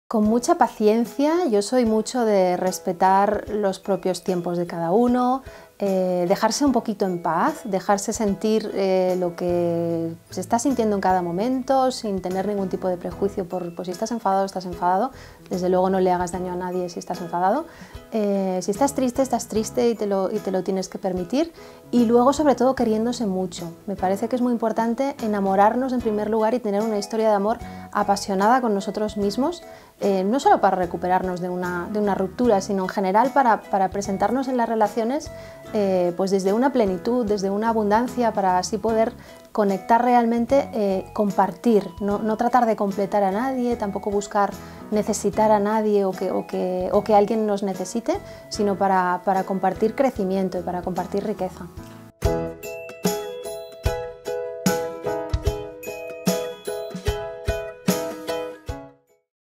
Rocio Carmona Lo que sucedió cuando me rompiste el corazón Romper Corazon
Rocío Carmona nos cuenta cómo cree que ella que se puere recuperar una persona al que le rompen el corazón, uno de los temas principales de su nueva novela, "Lo que sucedió cuando me rompiste el corazón".